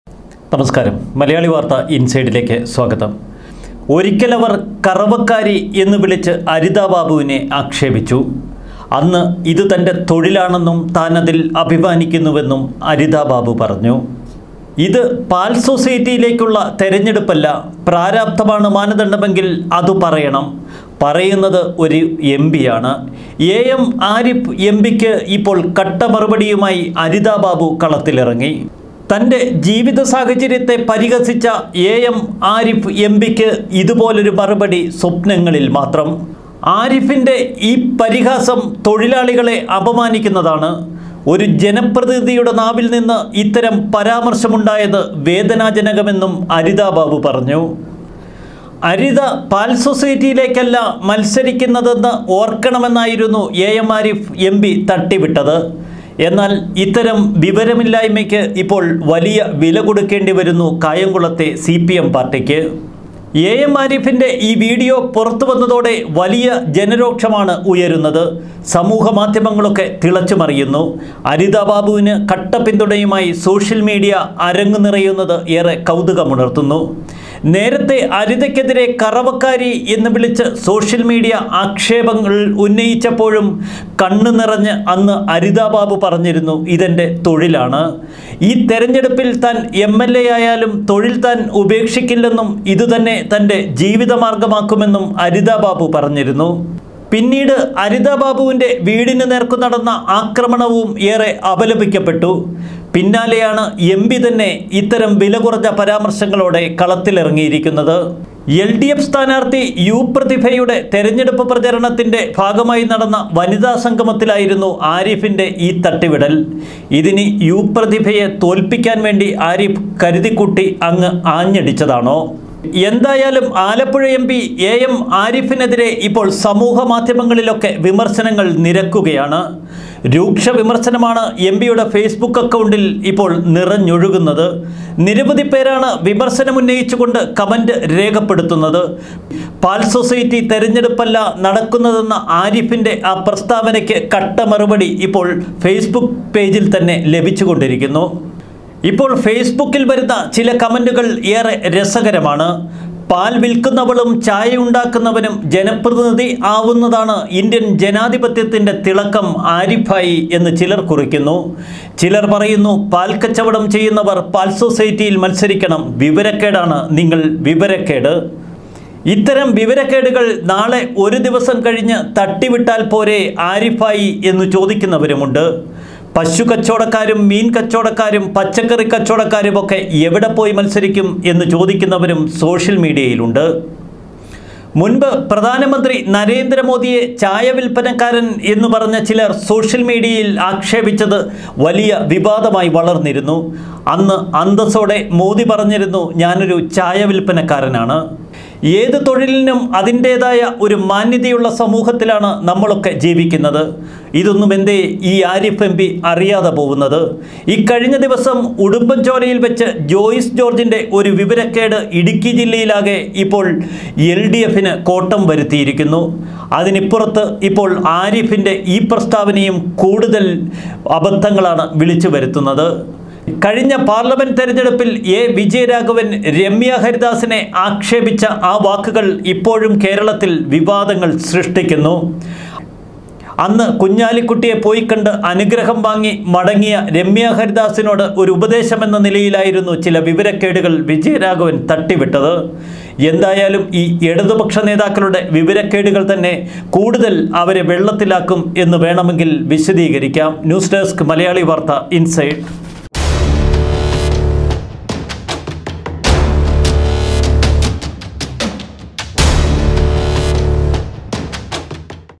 Aritha Babu അരിതാ ബാബുവിനെ സോഷ്യൽ മീഡിയ ഇങ്ങെടുത്തു.
"പാൽ വിൽക്കുന്നവളും,
ചായ ഉണ്ടാക്കുന്നവനും
ഇന്ത്യൻ ജനാധിപത്യത്തിന്റെ തിളക്കം.
ആരിഫിനെ ഓടിച്ചു..."
Aritha Babu